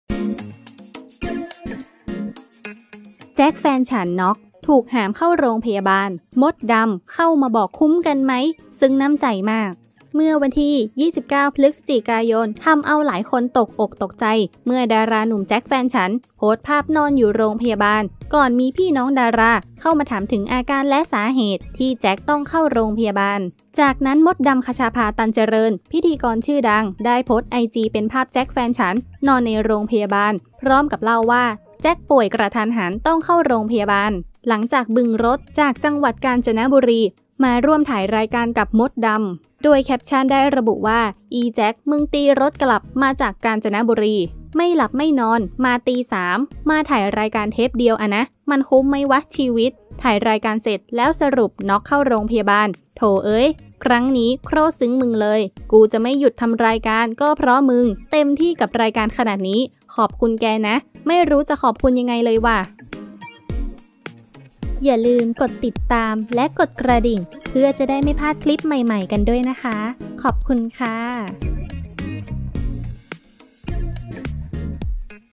‘แจ๊ค แฟนฉัน’น็อก! ถูกหามเข้ารพ. มดดำ เข้ามาบอกคุ้มกันมั้ย ซึ้งน้ำใจมาก
เมื่อวันที่ 29 พ.ย.63 ทำเอาหลายคนตกอกตกใจ เมื่อดาราหนุ่ม ‘แจ๊ค แฟนฉัน’ โพสต์ภาพนอนอยู่ในโรงพยาบาล ก่อนมีพี่น้องดาราเข้ามาถามถึงอาการและสาเหตุที่แจ๊คต้องเข้าโรงพยาบาล จากนั้นมดดำ คชาภา ตันเจริญ พิธีกรชื่อดัง ได้โพสต์ไอจีเป็นภาพแจ๊ค แฟนฉัน นอนในโรงพยาบาล พร้อมเล่าว่า แจ๊คป่วยกระทันหันต้องเข้าโรงพยาบาล หลังจากบึ่งรถจากจากจ.กาญจนบุรี มาร่วมถ่ายรายการกับมดดำ
     โดยแคปชั่นระบุว่า “อีjackมึงตีรถกลับมาจากกาญจนบุรี ไม่หลับไม่นอน มาตี 3 มาถ่ายรายการเทปเดียวอะนะ มันคุ้มไหมวะ ชีวิต ถ่ายรายการเสร็จ แล้วสรุปน็อกเข้าโรงพยาบาล โธ่เอ๋ย ครั้งนี้โคตรซึ้งมึงเลย กูจะไม่หยุดทำรายการก็เพราะมึงเต็มที่กับรายการขนาดนี้ ขอบคุณแกนะ ไม่รู้จะขอบคุณยังไงเลยวะ